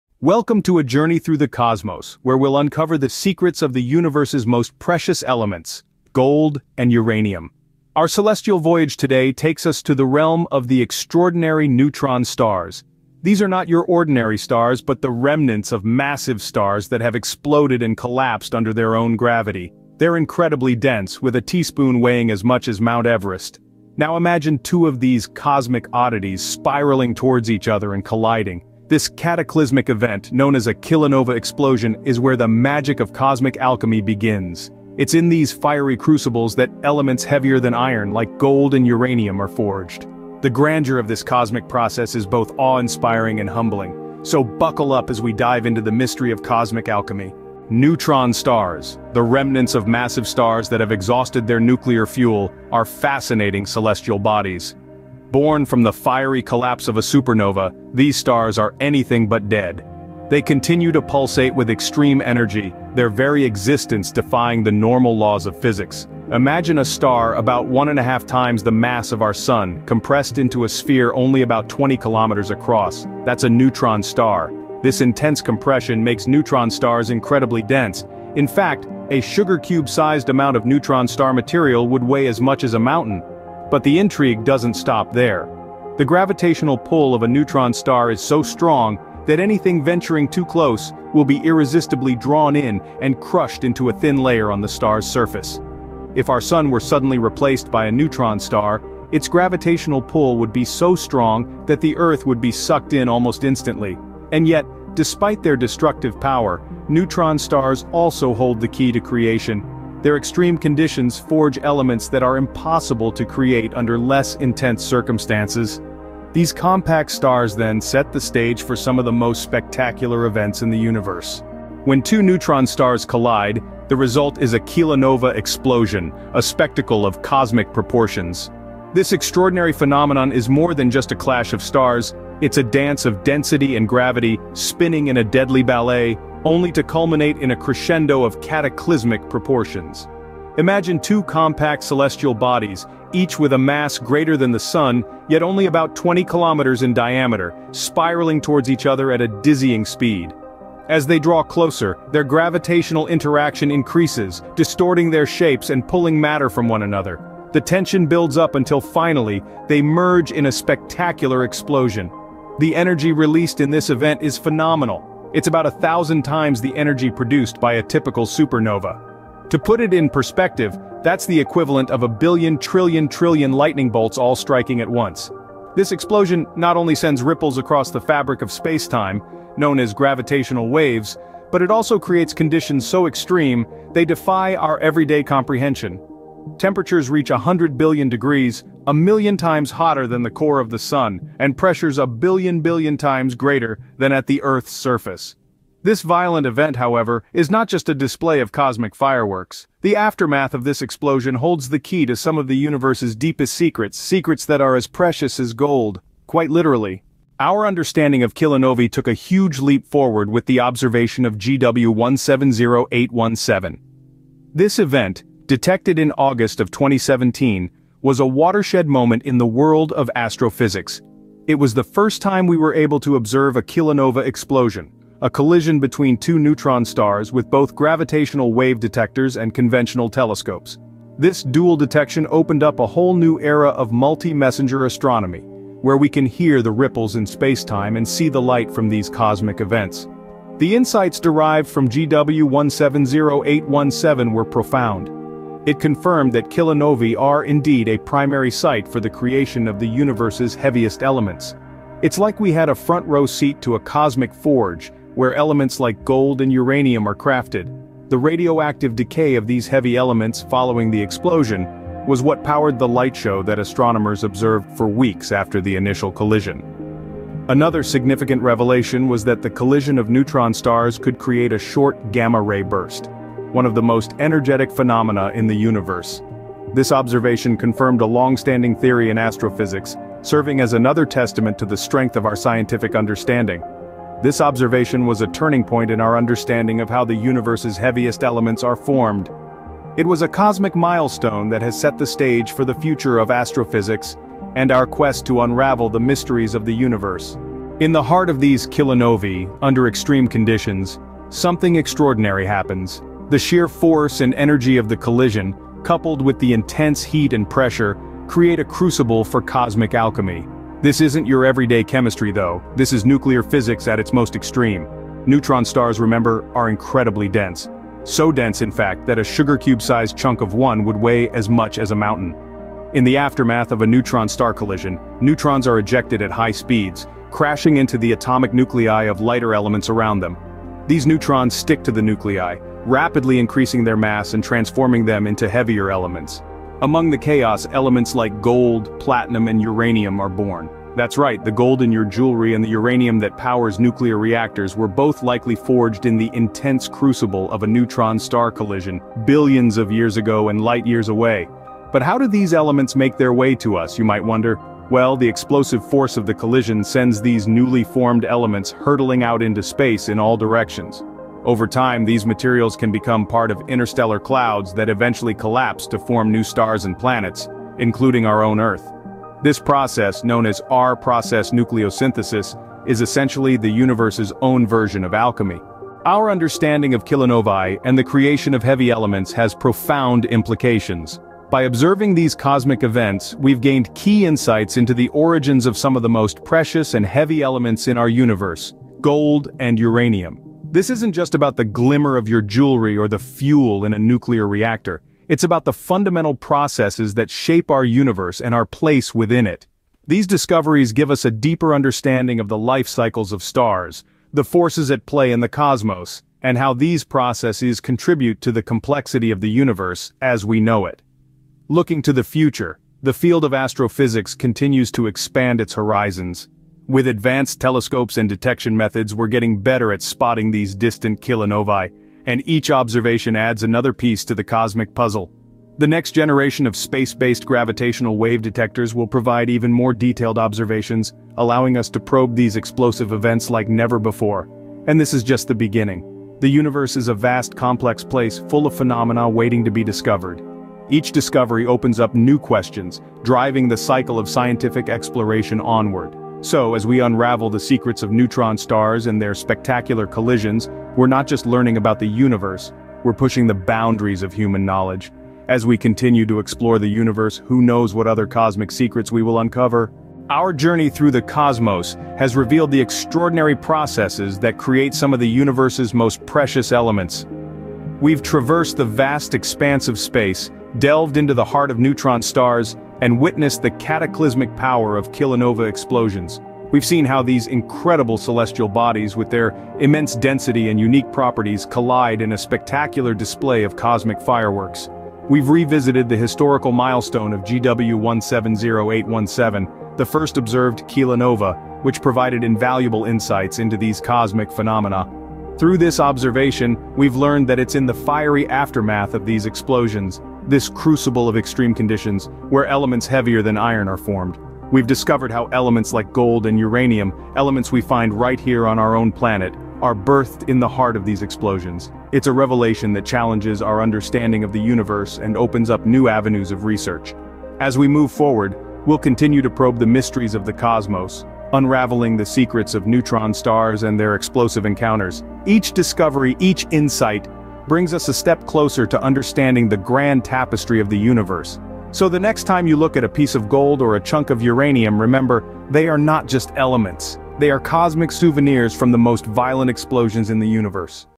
How Heavy Elements Gold and Uranium are Formed In Outer Space
Explore the cosmic wonder of kilonova explosions, where crashing neutron stars forge elements like gold. This educational video delves into neutron stars, the phenomenon of kilonova explosions, and the groundbreaking observation of GW170817. We uncover how these celestial events create heavy elements, reshaping our understanding of the universe. Perfect for astronomy enthusiasts and those curious about the grand cosmic processes.